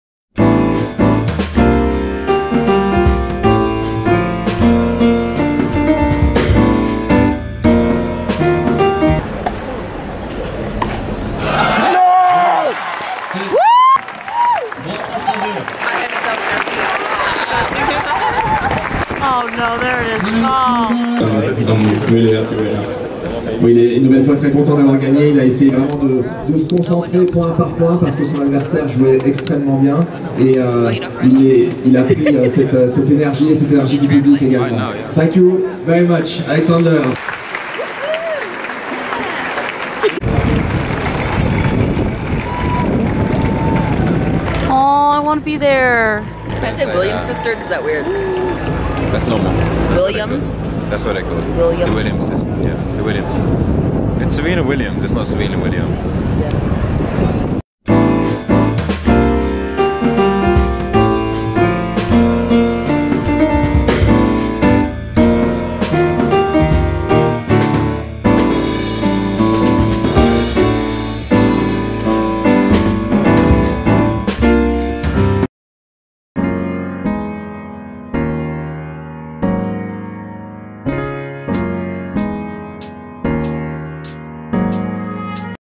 ZVEREV PULLING IN THE WIN AT FRENCH OPEN! ♡ | Paris French Open Vlog
El and I got to witness Zverev win the third round at the Paris French Open 2018. Phil was not so happy.

♡ katelynn cassidee rae

ABOUT KATELYNN CASSIDEE RAE:
Hi! I take you on my adventures and chat about my life in hopes of inspiring you to live your best life! ☺